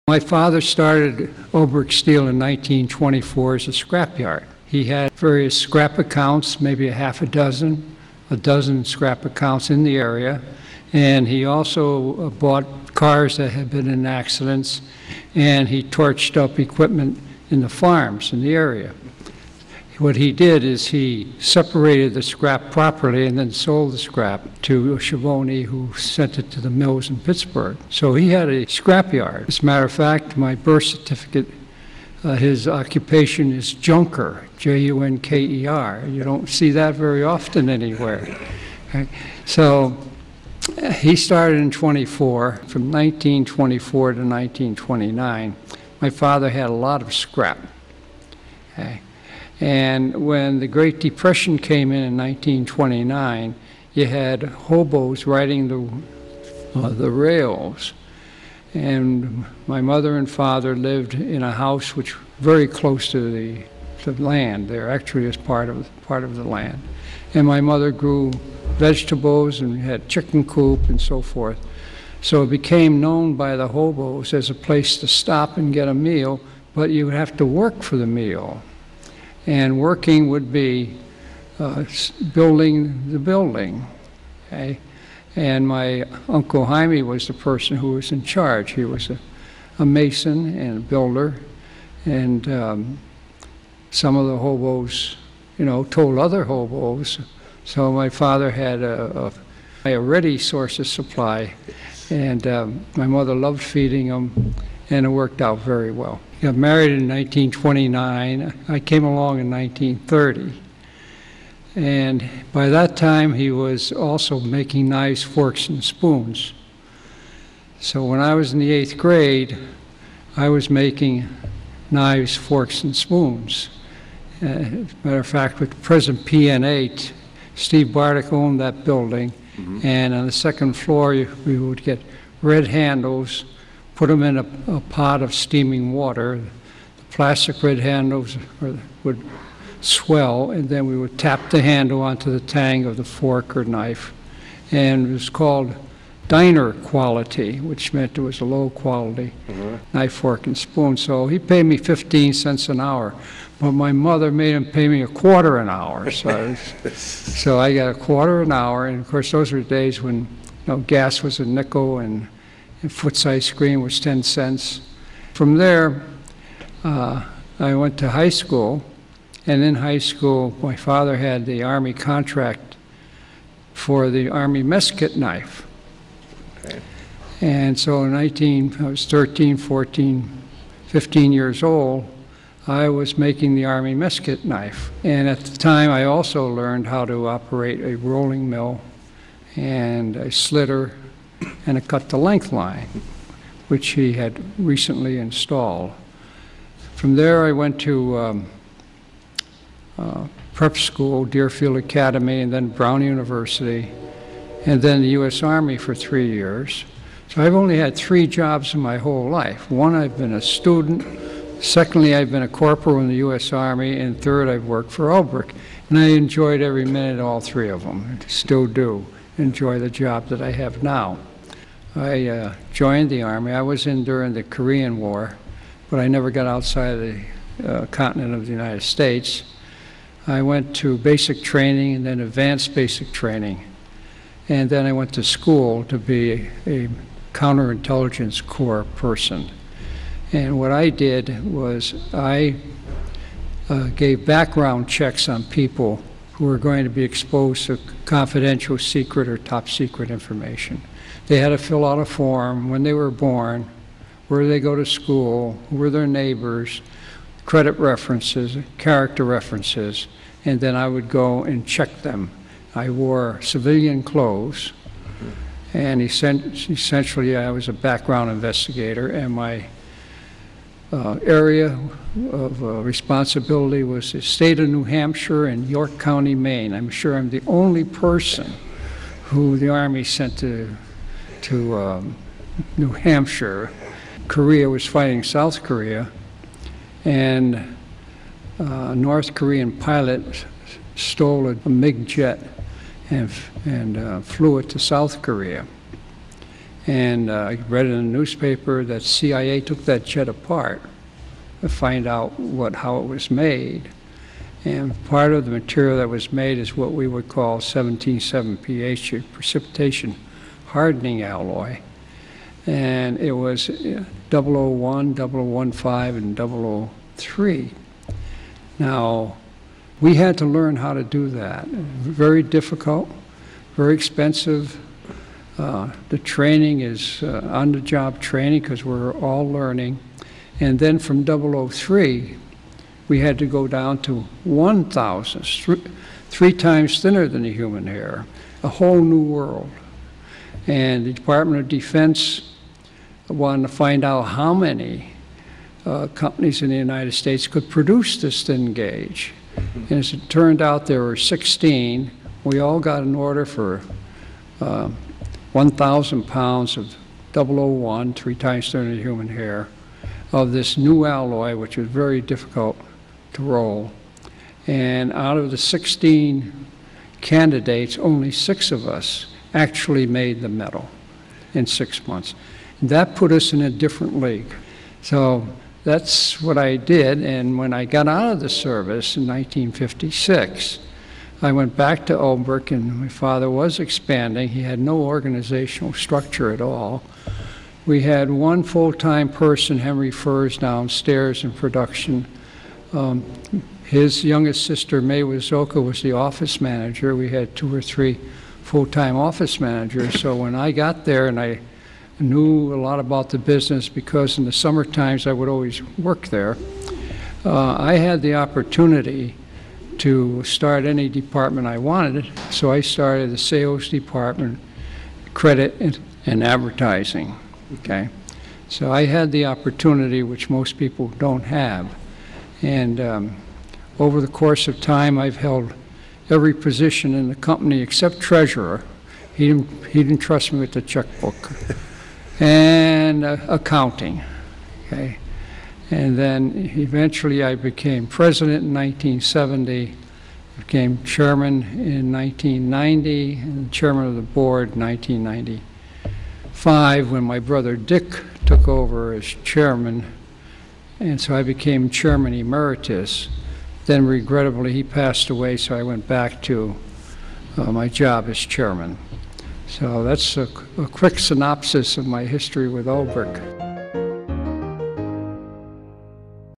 From the Archives: Fred Ulbrich's Story is a Wallingford Story
MoreThanTV produced at WPAA-TV from legacy footage. Source files Jan 2013